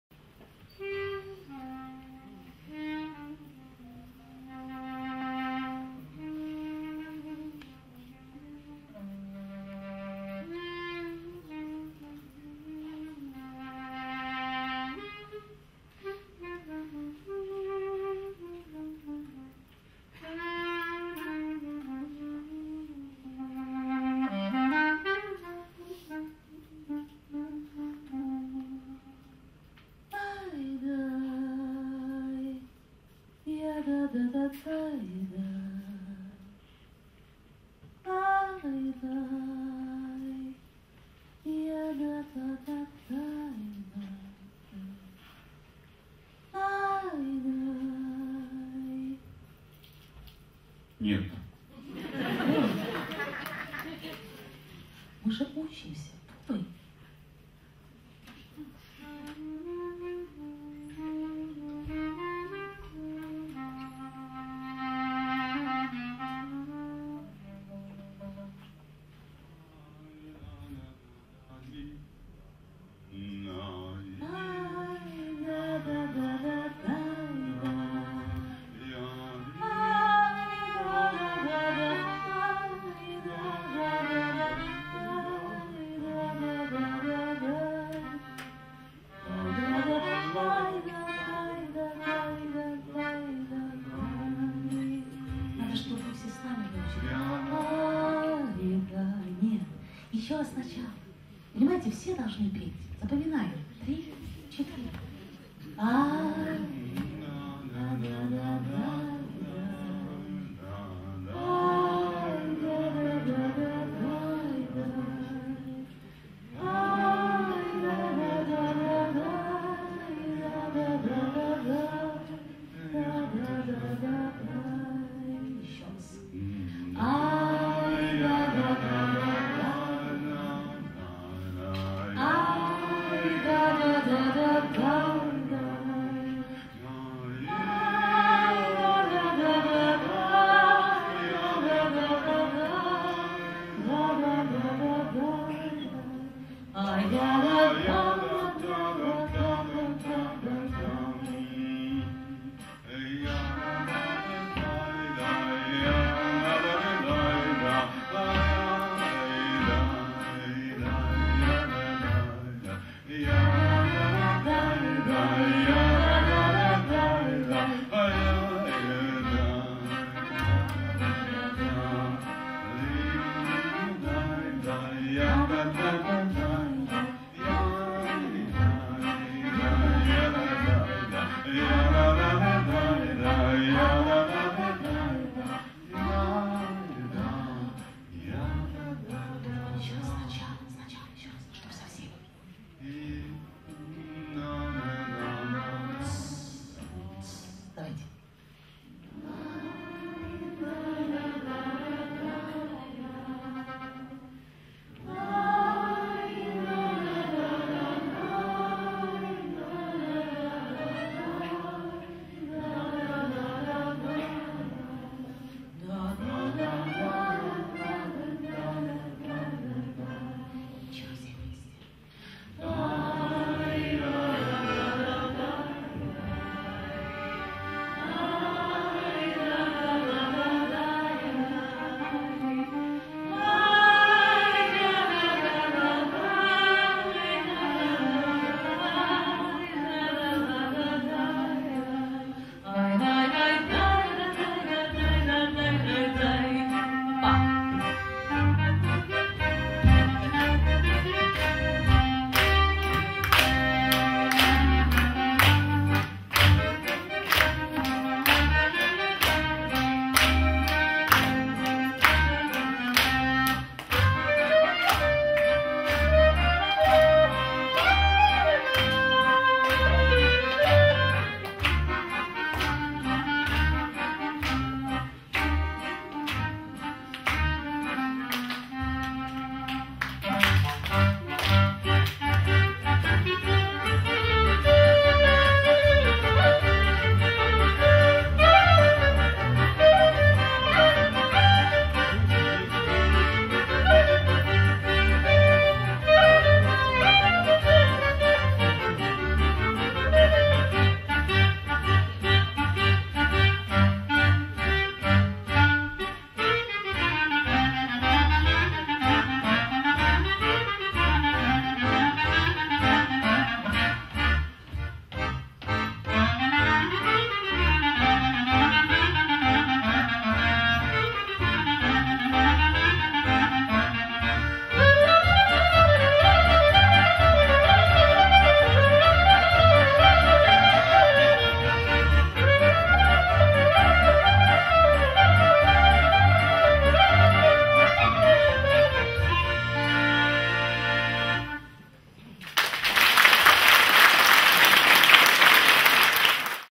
Владимир Верягин. Харьков Клезмер Тег 2018-10-14 Гала-концерт
Харьков Клезмер Тег 2018. Гала-концерт.
Видео: Броня Громова.